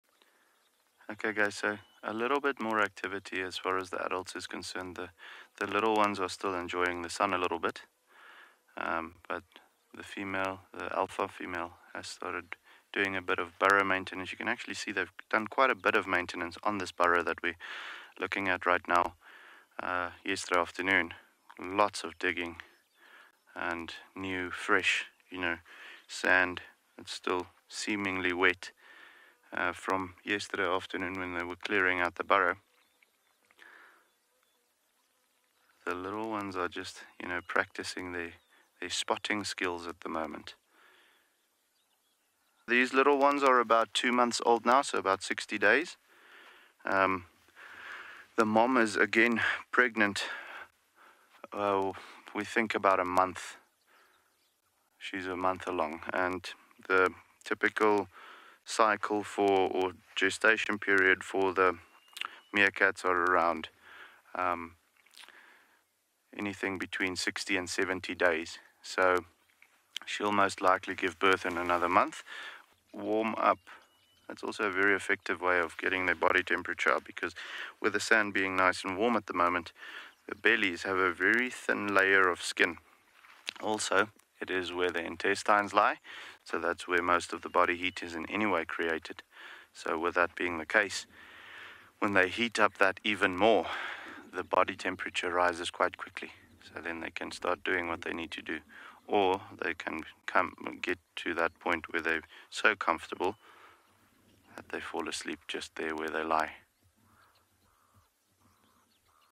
Cute little pups
WE had a visit to the Rockstar meerkat Den to catch up on the new pups which have joined the mob with Mauritz and Craig in Tswalu Kalahari Reserve.

Drive: Sunrise -19 January 2021